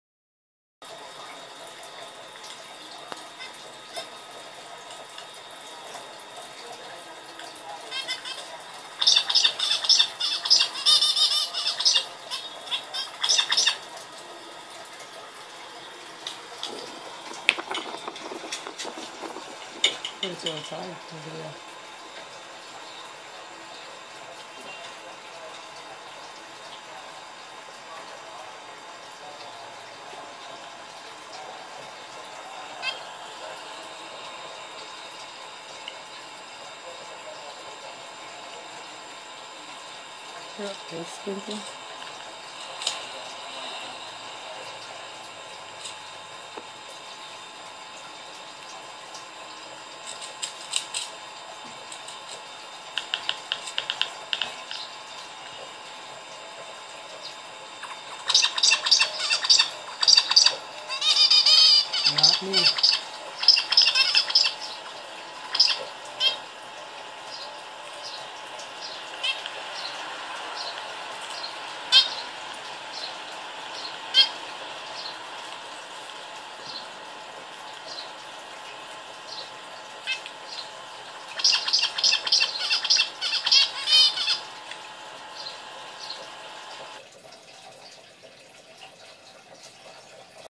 Zebra finch watching video
Zebra finch watching video on my phone...scout is a crazy lil fella lol